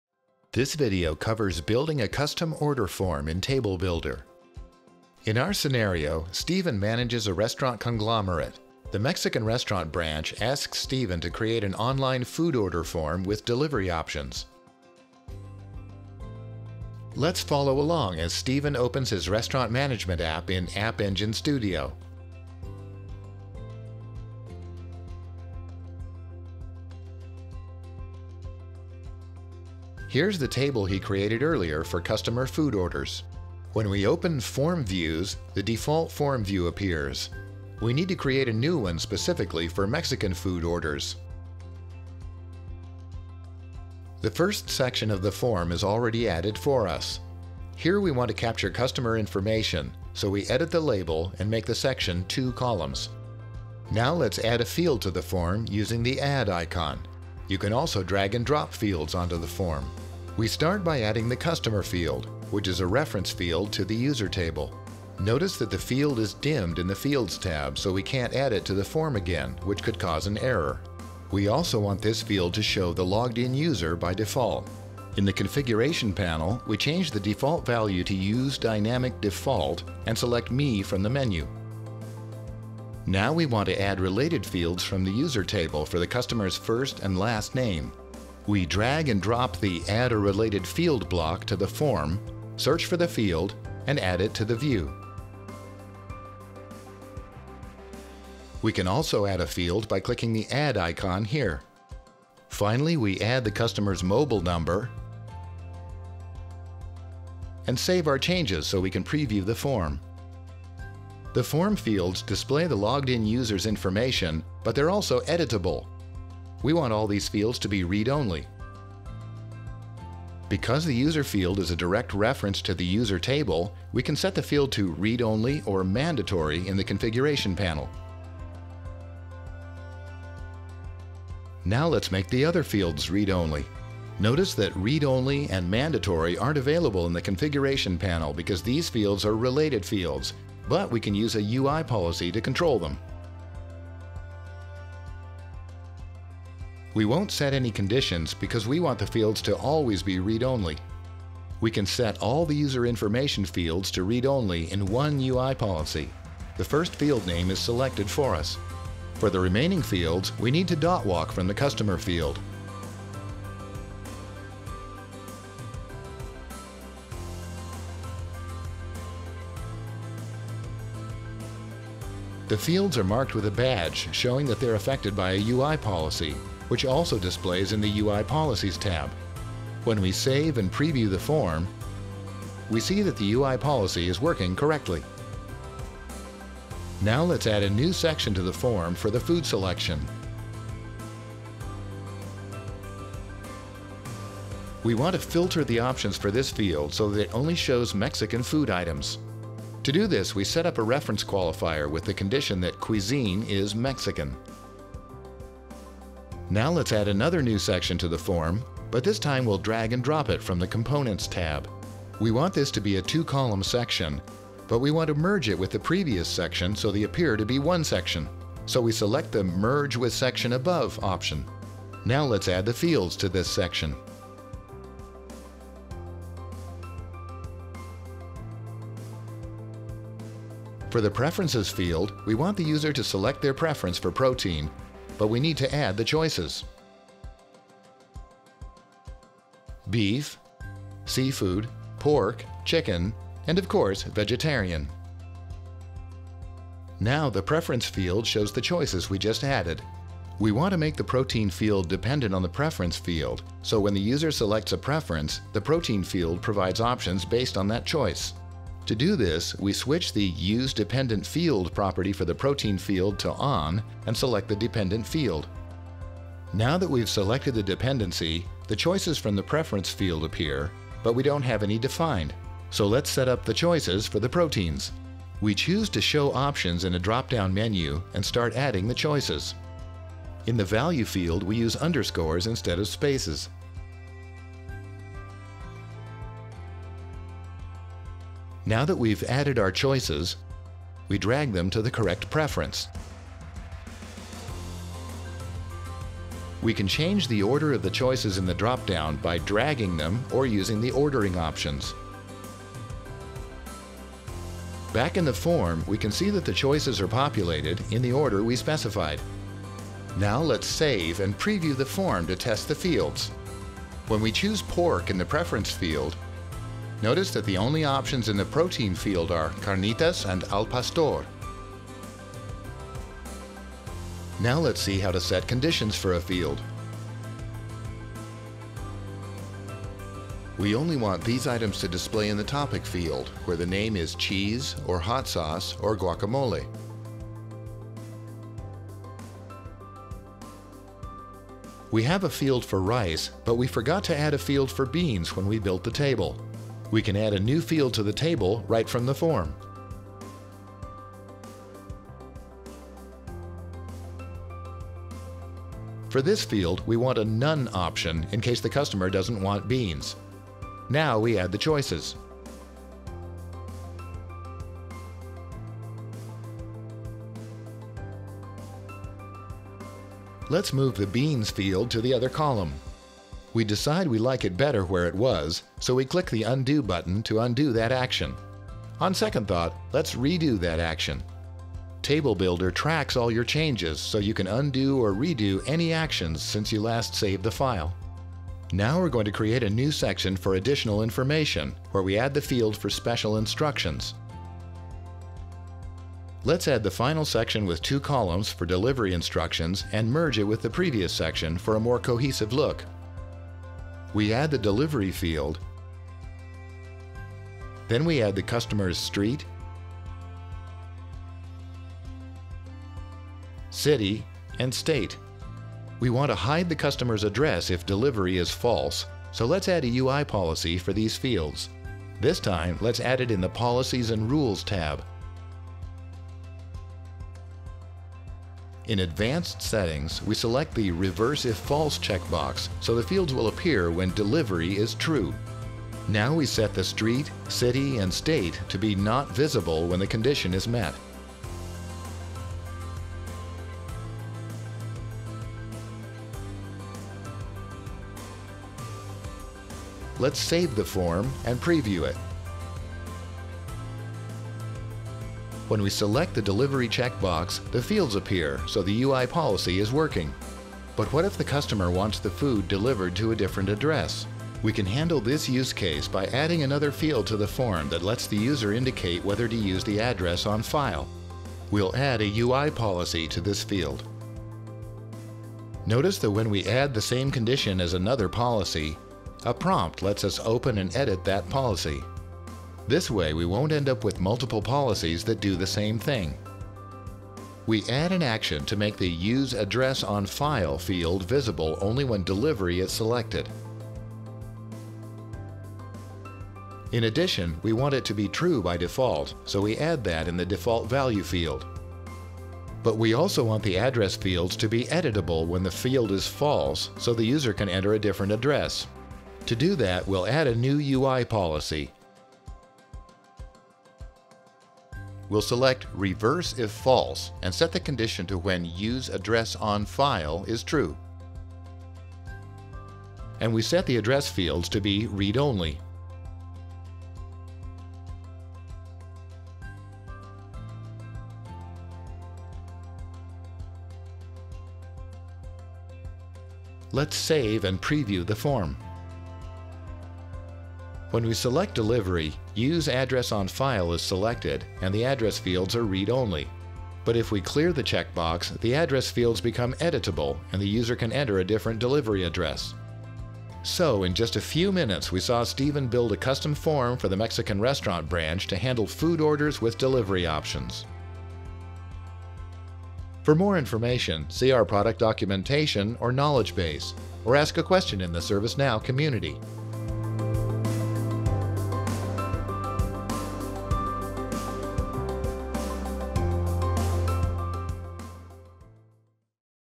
Table Builder | Creating a custom order form
Shows you how to build a custom order form in Table Builder.

Role required: admin or users with specific permissions

This video covers:
00:00 Intro
00:29 Creating a new form view
00:53 Adding fields to the form
01:11 Configuring a default value for a field
01:51 Make fields read only or mandatory
02:11 Making related fields read only with UI policies
03:07 Adding sections to the form
03:14 Filtering field options with a reference qualifier
03:26 Merging sections in a form
03:50 Adding choices to a field
04:10 Controlling menu options with a dependent field
05:23 Setting conditions for a field
05:38 Adding a field to the table from a form
06:07 Undo/Redo actions
07:54 Editing a UI policy

This video was recorded in the San Diego release. It may apply to other releases as well.